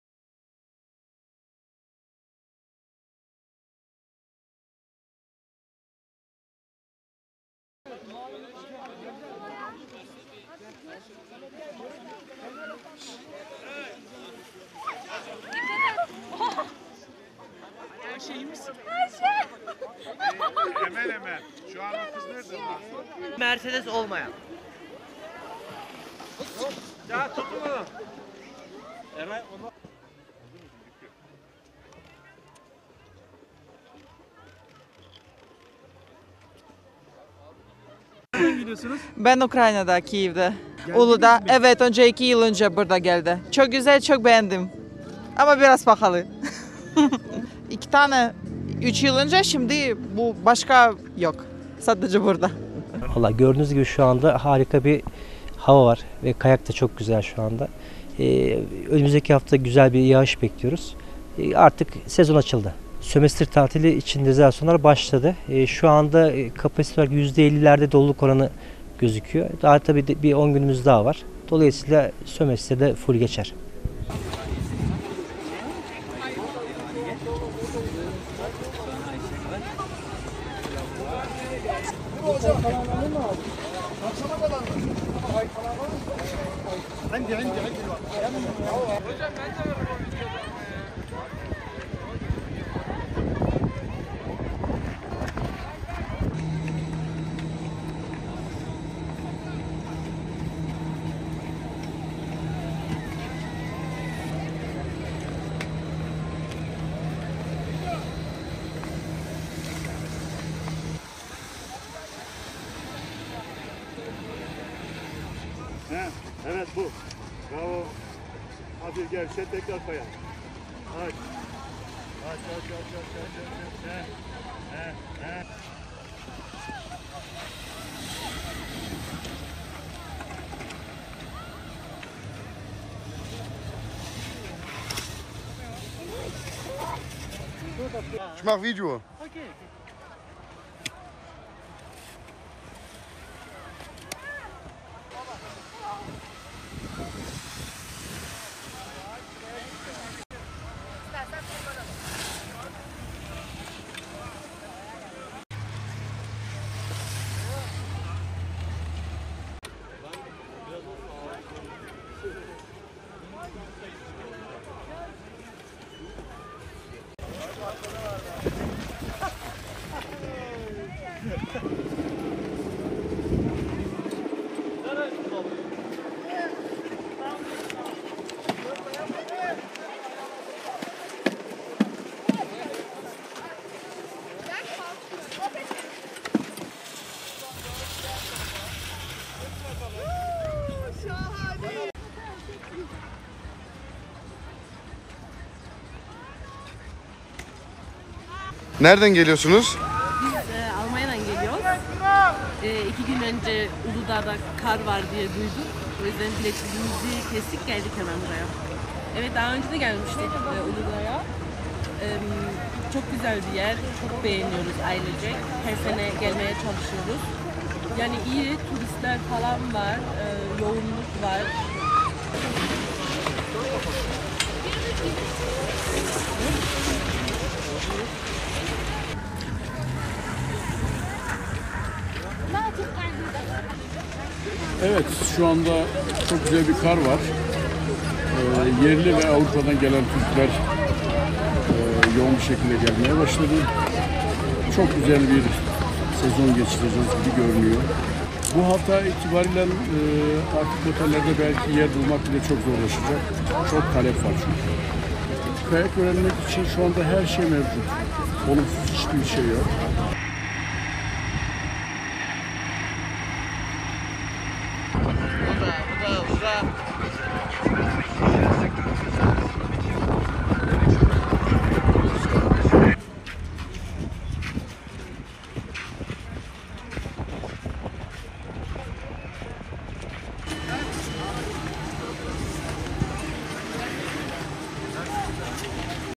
Uludağ'a akın var!
Uludağ’da kar yağışının etkisini arttırması nedeniyle binlerce yerli ve yabancı turist pistlere akın etti.